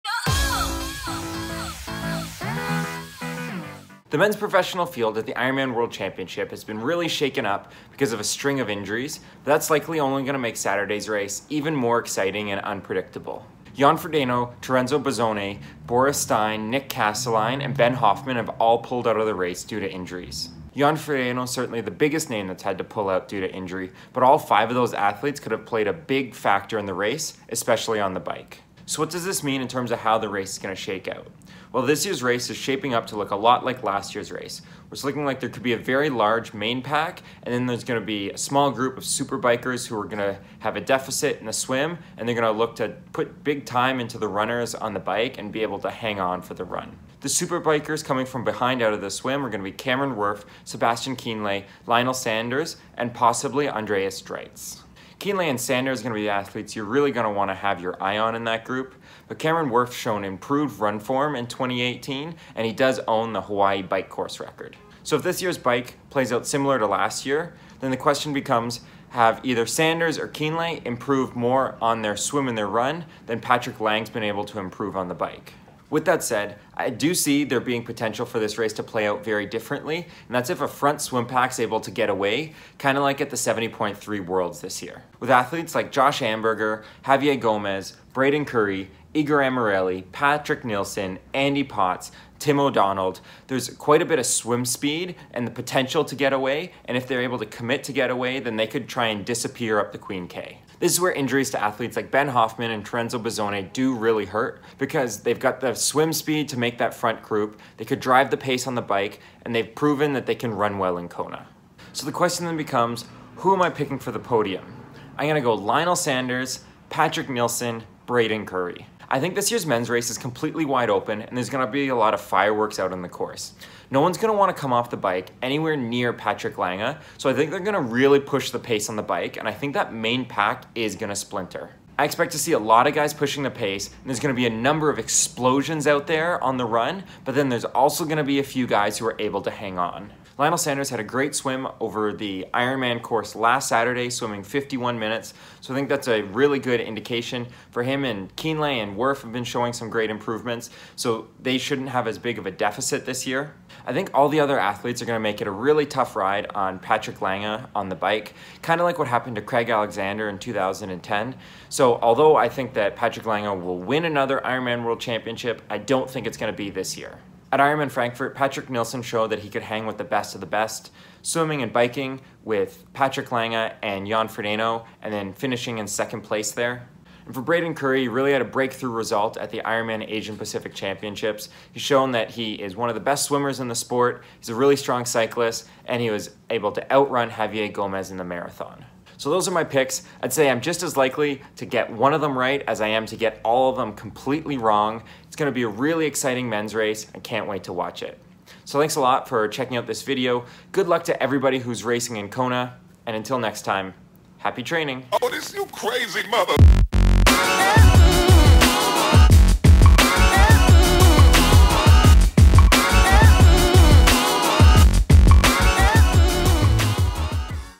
Ironman World Championship Men's Race Preview - Vlog 104
The men's race in Kona has been shaken up due to some injuries, but that's only made this year's race even more wide open. Nobody with intentions of winning is going to want to come off the bike anywhere near Patrick Lange, so there's the potential for a lot of fireworks on the bike, not just from Sanders, Kienle, and Wurf, but also from the main pack. It's going to be a dynamic race that should be exciting right to Ali'i Drive.

Good luck to everyone racing in Hawaii.